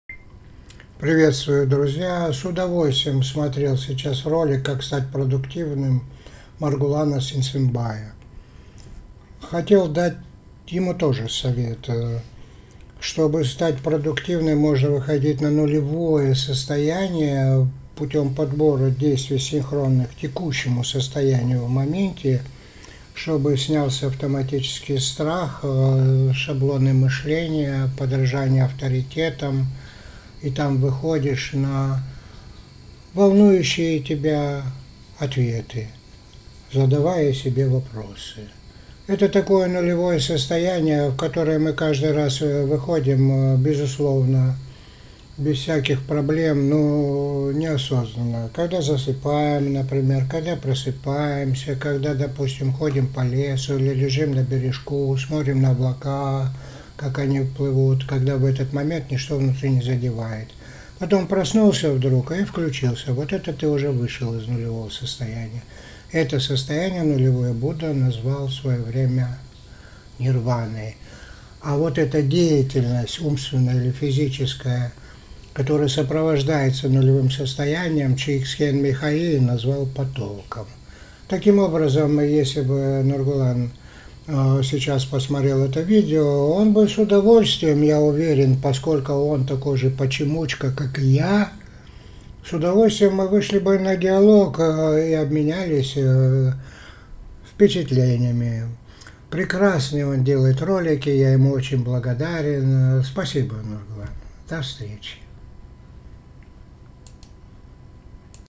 Знает ли Маргулан Сейсембай Метод Ключ? Как быть продуктивным. Dr Hasai Aliev MD. The method "Key"
Д-р Хасай Алиев, Заслуженный врач, психолог, писатель, художник говорит о том, что с методом Ключ быть продуктивным проще.